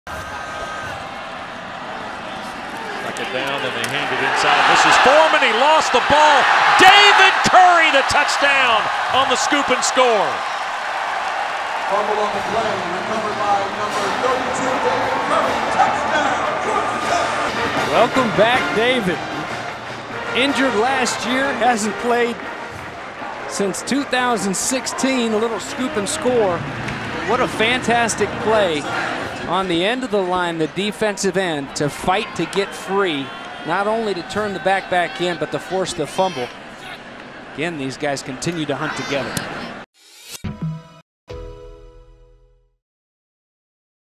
Georgia Tech Scoop & Score Cements Shutout vs. Alcorn State
Georgia Tech put together a dominant defensive performance against Alcorn State.  David Curry comes up with the fumble recovery and returns it for a touchdown during the impressive performance in this ACC Must See Moment.